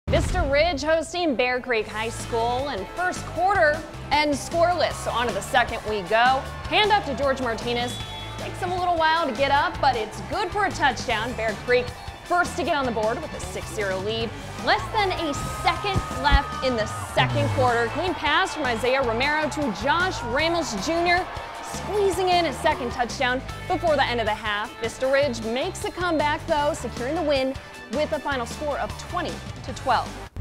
Vista Ridge comes back to pick up win over Bear Creek
Vista Ridge wins 20-12 over Bear Creek.